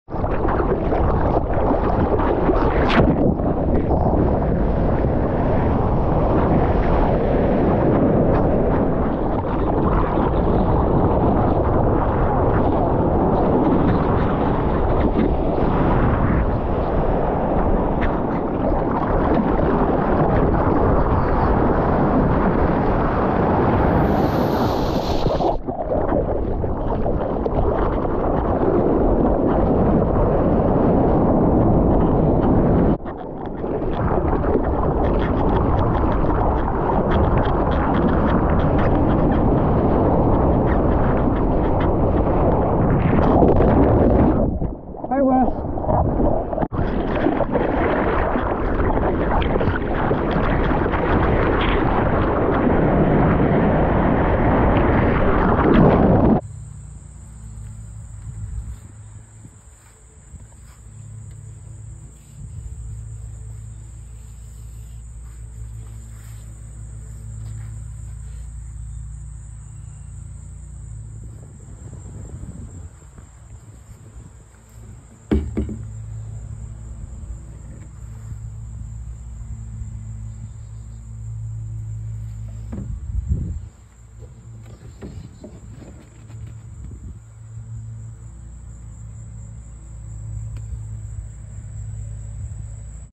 Fun surf day. WRV vapor 6’0”
I’ve had this board for about a year now. At the end of the video you will see all the dings that have came along with the past year. Still a fun board to ride.